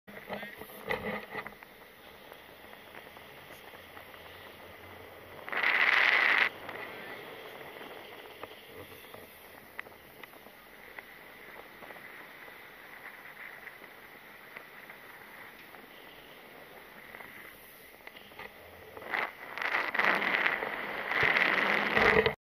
Terri paragliding Murren Switzerland 2018
Terri flying paraglider in Switzerland